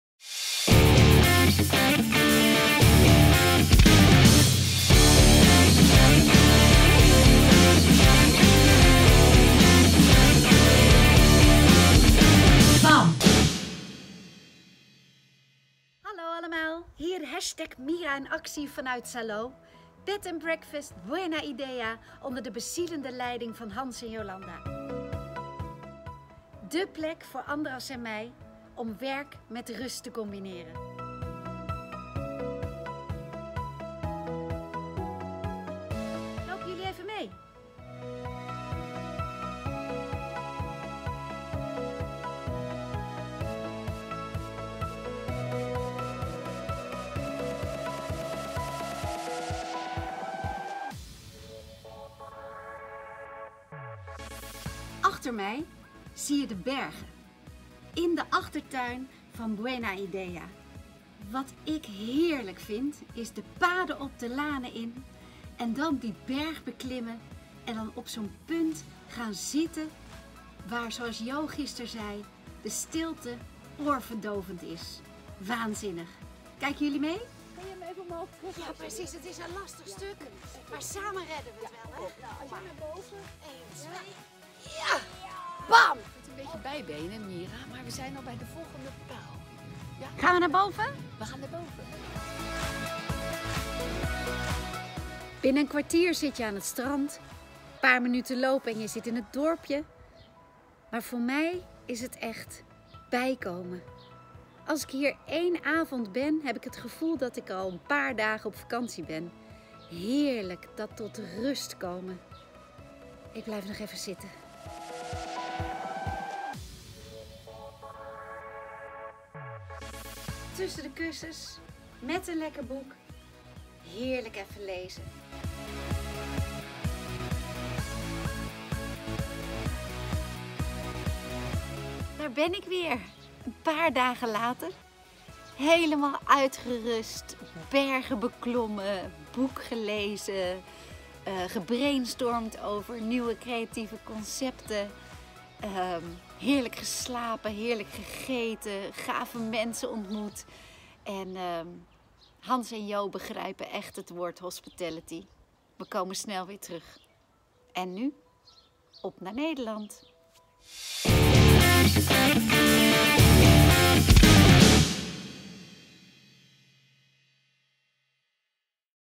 Myra in Actie Deel 18 Special Holiday:work edition
Deel 18 Myrainactie BAM vlog Special edition combinatie werk & rust bij Bed & Breakfast Buena Idea Myrainrelaxmodus vakantie werkvakantie uitrusten joinxalo xalo jalon costablanca buenaidea bedandbreakfastbuenaidea bedandbreakfastjalon bedandbreakfastxalo foto’s: Andras Fictoor